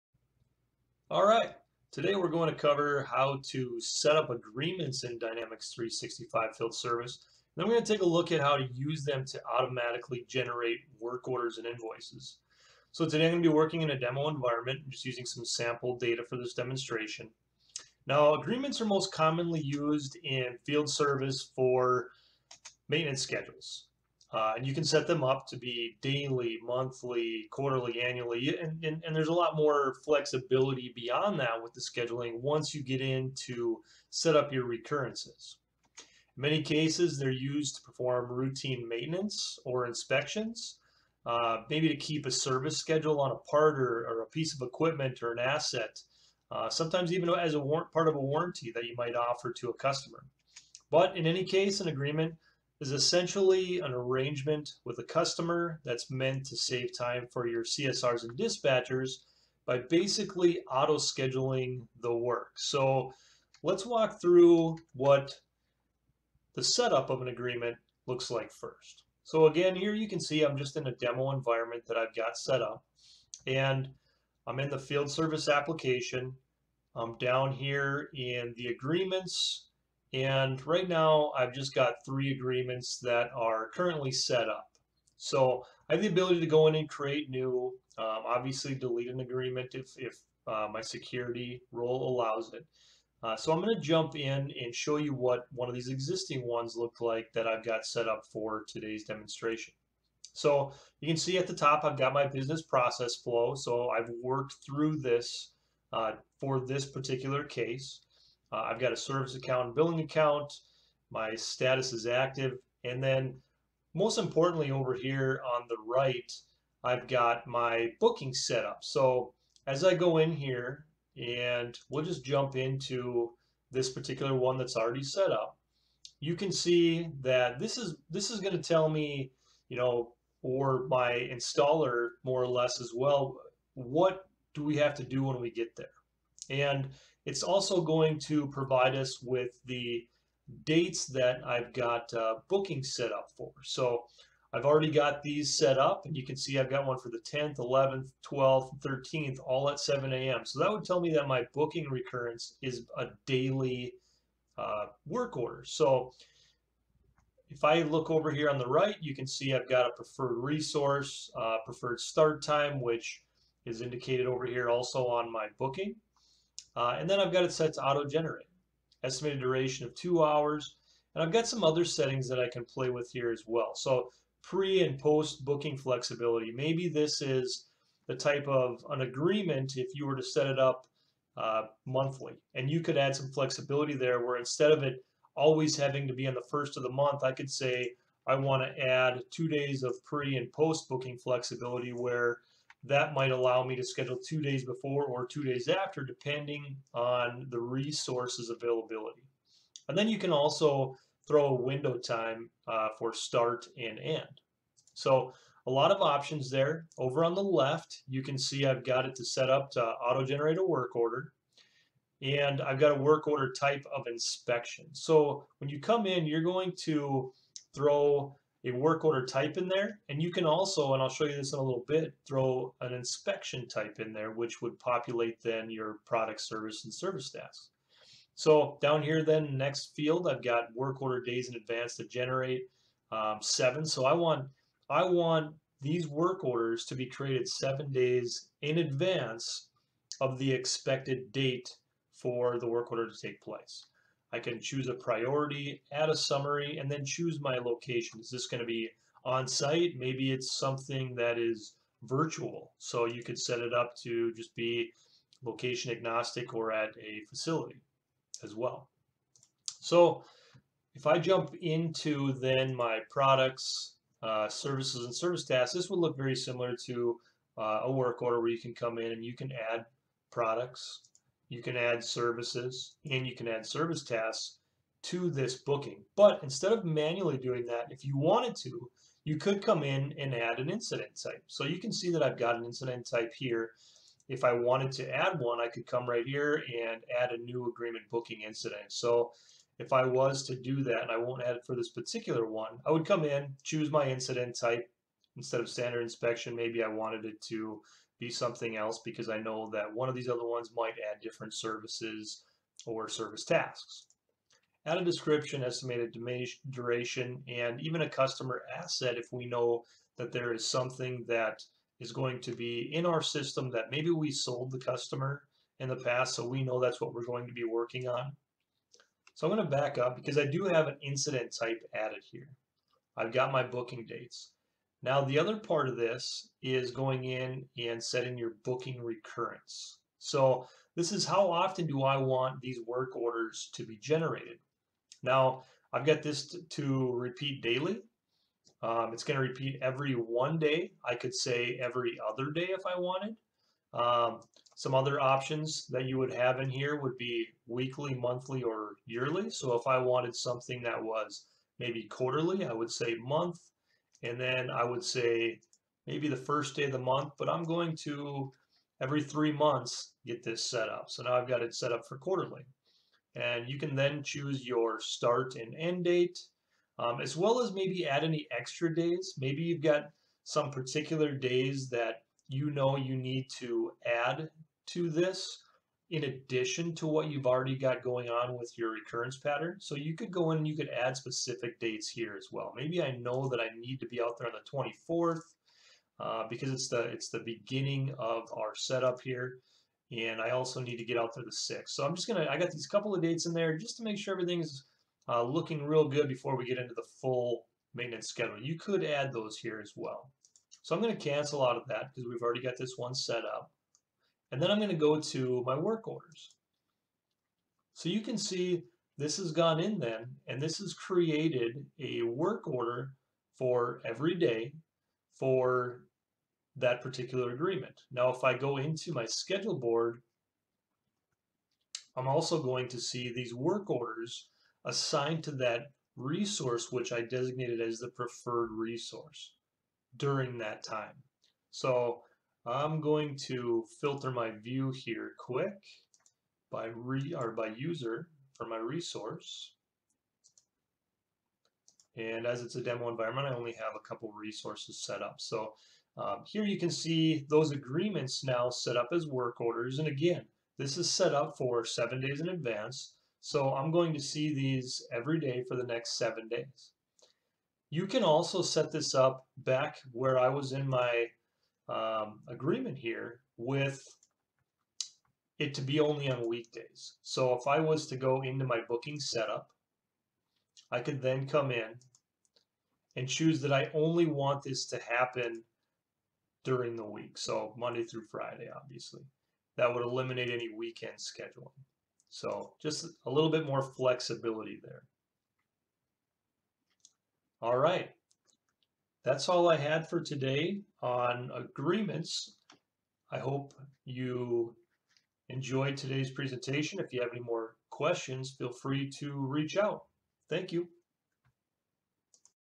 Field Service Agreements in Dynamics 365
What are they for? And how to set them up.
Skip to 1:10 minute mark to get right into the demo.